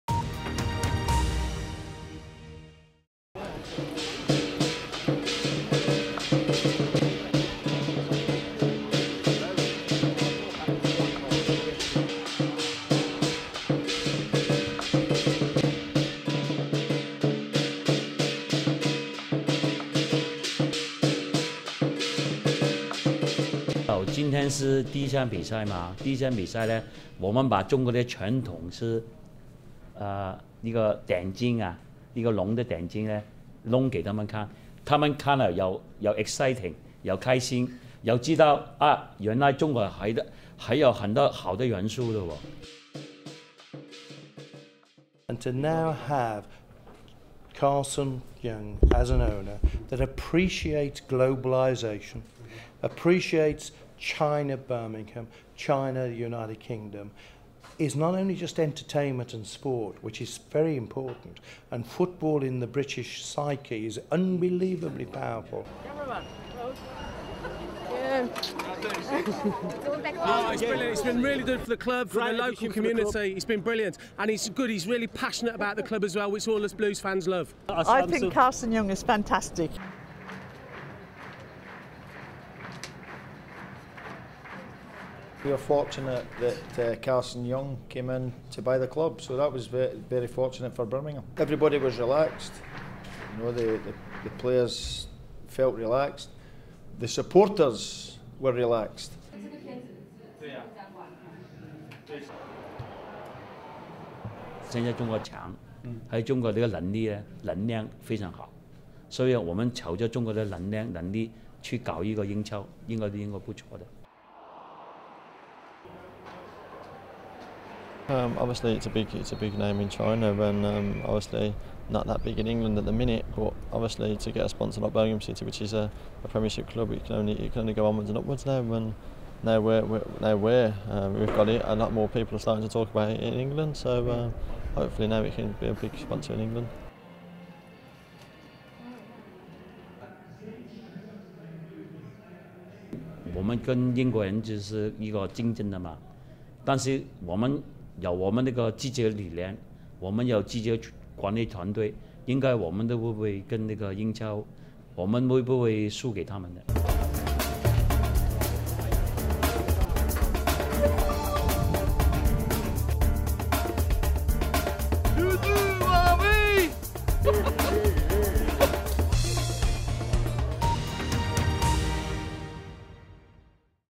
BBC中文网视频：走访英超伯明翰俱乐部
8月21日，英超新赛季开始后的第二轮比赛中，伯明翰队首场主场迎战布莱克本，开赛前绿茵场上的舞龙表演引起球迷极大兴趣。

去年香港商人杨家诚正式收购伯明翰足球俱乐部之后，大力开拓中国市场。杨家诚在接受BBC英伦网采访时，很有兴致地谈到从赛前舞狮、到引入中国品牌的球衣给伯明翰带进的中国元素，并表示要用中国人务实稳健的作风来管理伯明翰，而不是一蹴而就。他的观点得到了包括主教练麦克利什和伯明翰市长维特比的认同。

伯明翰足球俱乐部始建于1875年。在近年的英超历史中，伯明翰队三起三落，频繁进出英超，上个赛季结束时位居中游，成绩不错。

伯明翰队的球迷们如何看待来自遥远东方的新"东家"？曾任苏格兰国家队教练的主帅麦克利什对老板印象如何？杨家诚本人又在商业竞争激烈的英超中胜算几何？